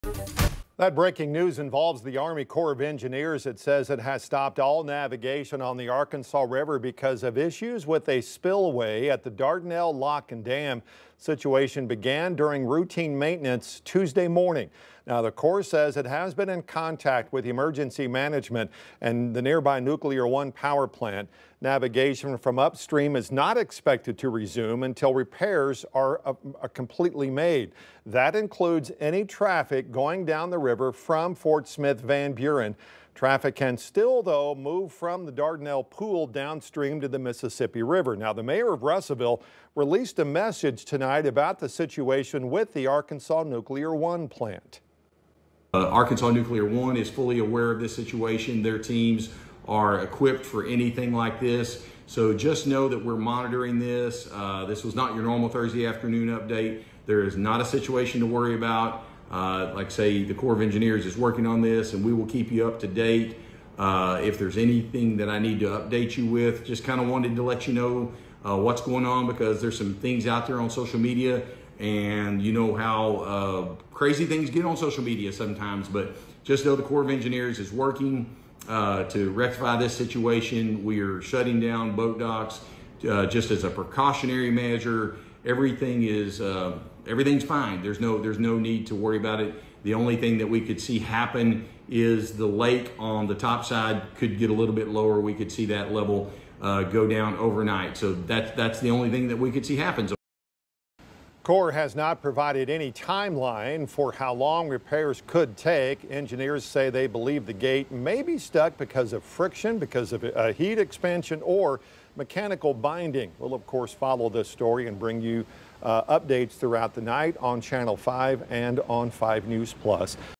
Repairs underway on stuck spillway gate at Dardanelle dam on the Arkansas River
The U.S. Army Corps of Engineers said the gate may be stuck due to friction, heat expansion, or mechanical binding.